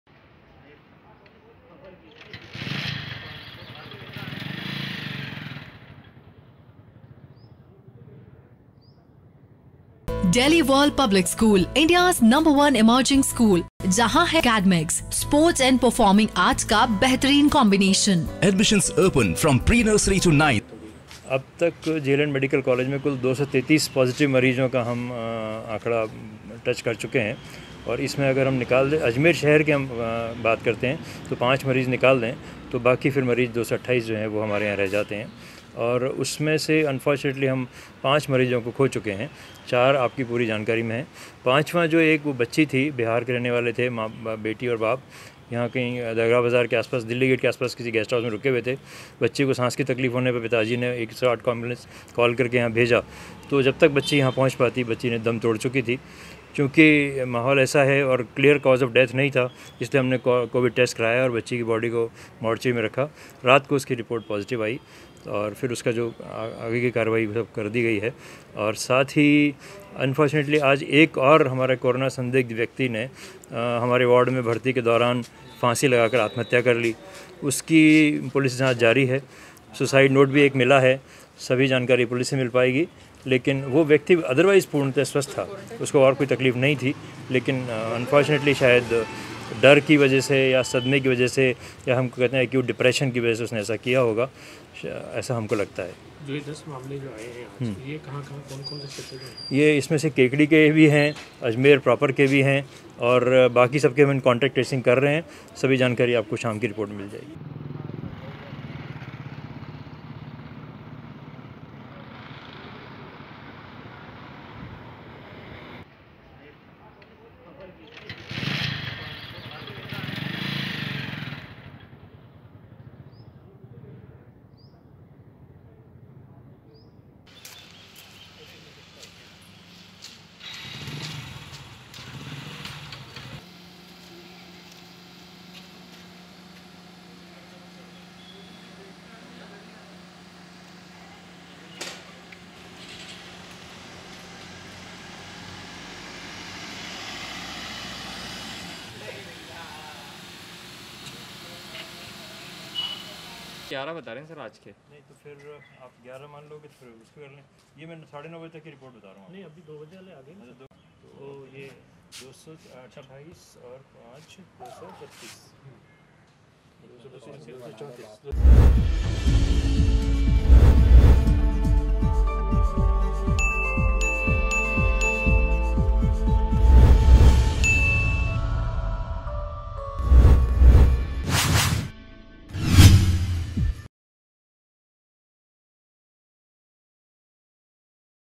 AJMER NEWS | सुबह 6 और दोपहर को 5 नए कोरोना पॉज़िटिव आए सामने | MTTV INDIA
अजमेर में सुबह 6 और दोपहर को 5 नए कोरोना पॉज़िटिव आए सामने
2 मरीज लॉ कॉलेज क्वारंटीन सेंटर से, 2 मरीज सेटेलाइट अस्पताल से और 1 मरीज प्लाइवुड फ़ैक्टरी से आया सामने
अजमेर में अब तक कोरोना से कुल 6 मौत हुई
जेएलएन अस्पताल में आत्महत्या करने वाले मरीज की रिपोर्ट कोरोना पॉज़िटिव आई